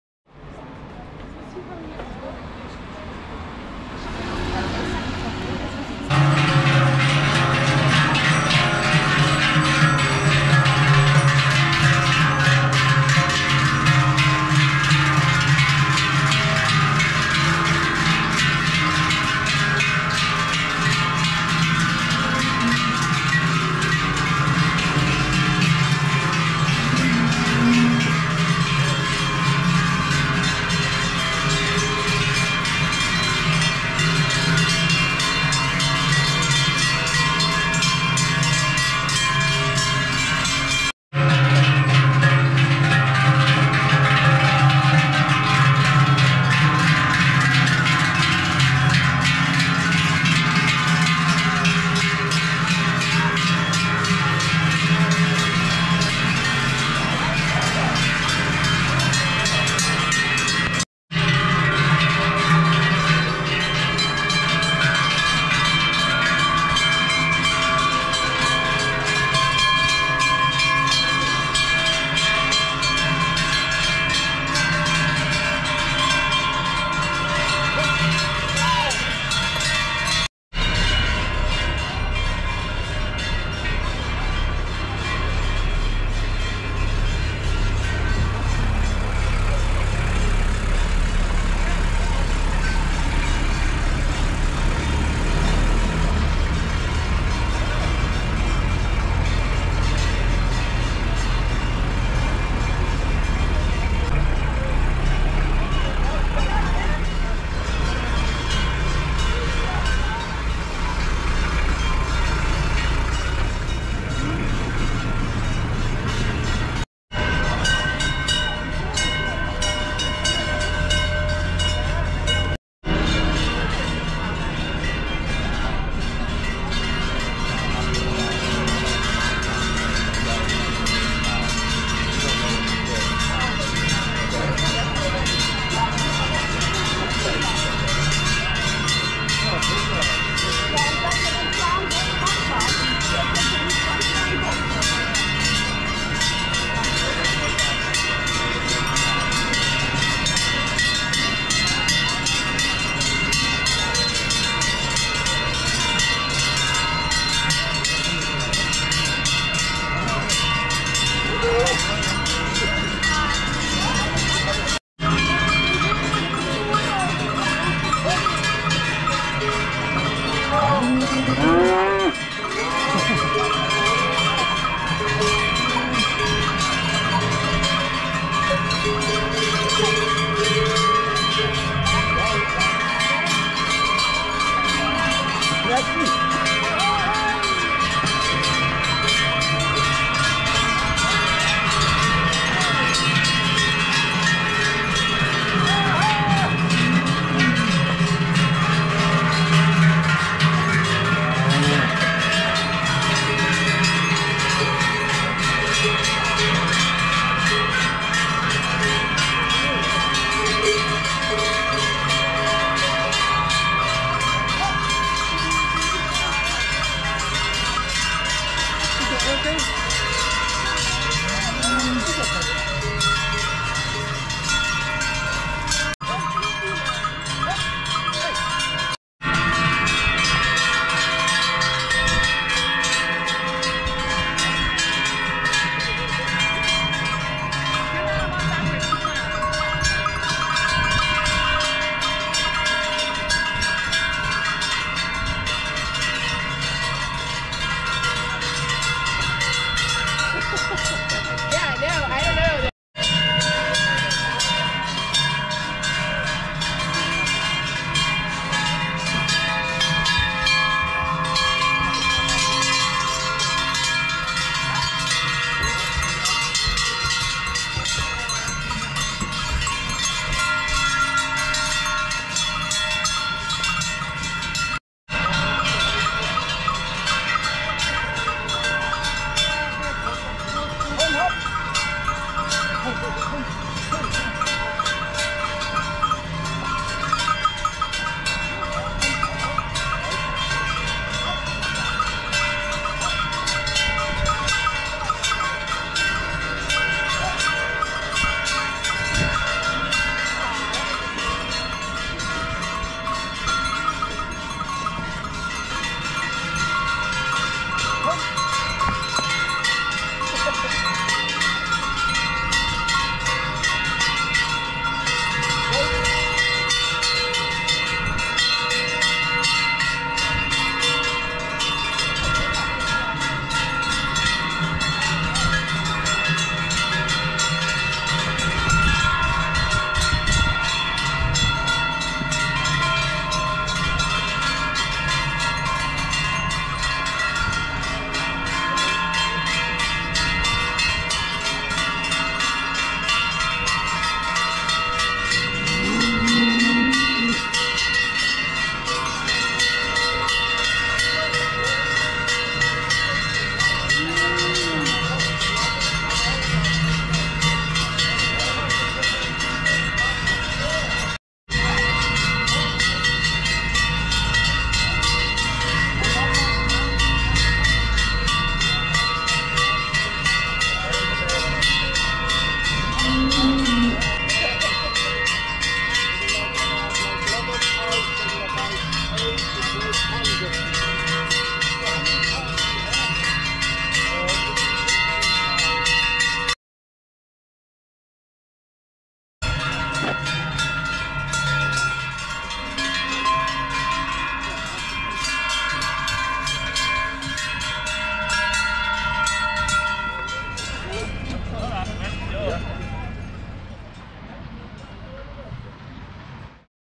Cow descent from mountains in Lauterbrunnen
One of those moments of pure joy.  Cow descent  in valley of Lauterbrunnen  . Thid is when the herd of cows march down from Alpine mountains  to valley barn. Bim , bam , bim. the bells tinkle in decorated cows as they lethargically  parade with their keepers in traditional dresses  At times collective moo of herd creates its own symphony . The trail gets splattered with Alpine pizzas ( dung) but nobody minds. A strange sense of joy runs through everyone witnessing this wonderful man-cattle relationship and bucolic tradition.